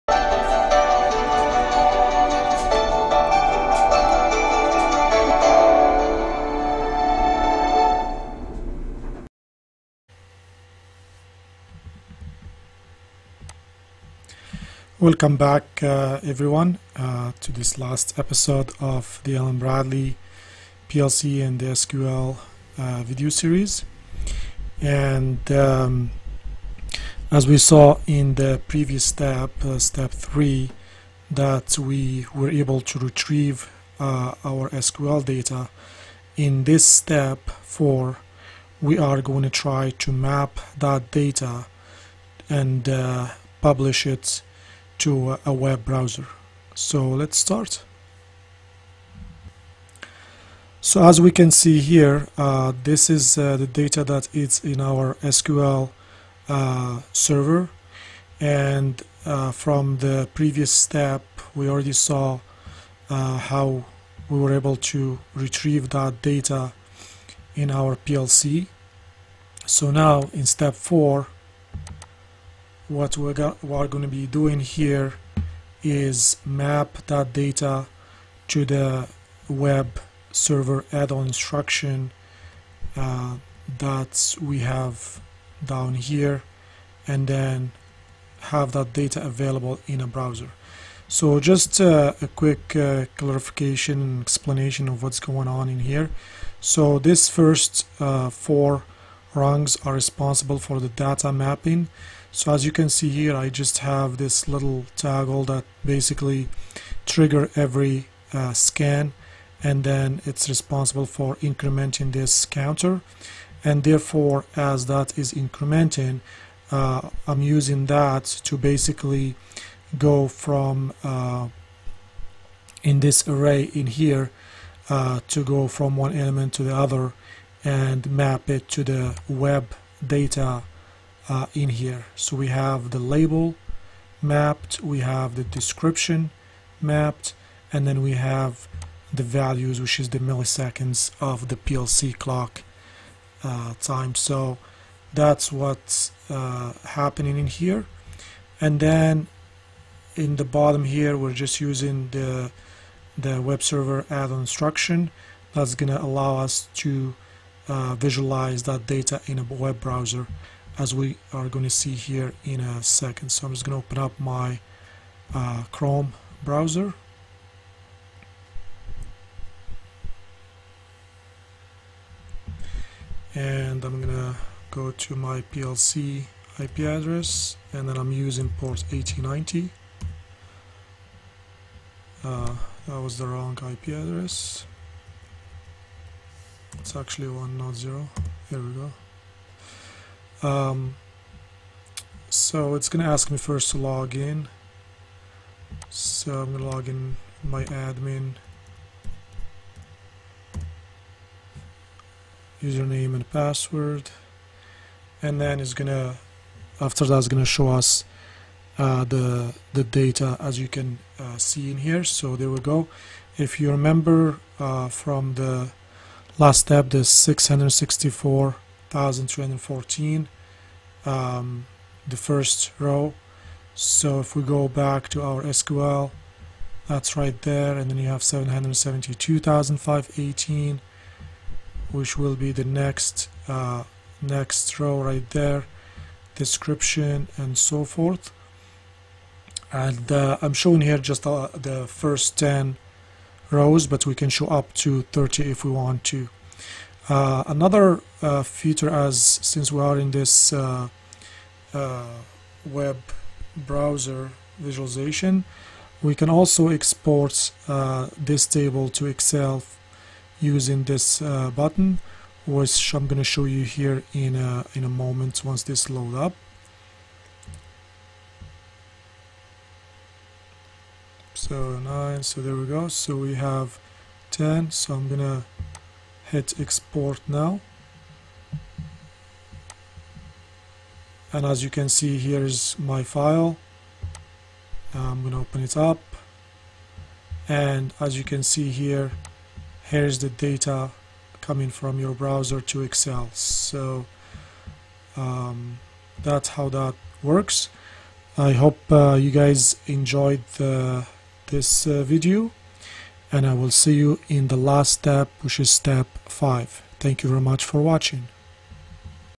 Rockwell PLC w/ SQL: Log & Publish -Step4
This is the fourth video of this play list where our ultimate goal is to log data to SQL from our PLC to retrieve later and then publish to a web browser. And in this step 4 we will discover how we publish our retrieved data from  SQL server to a web browser by using only our Rockwell PLC.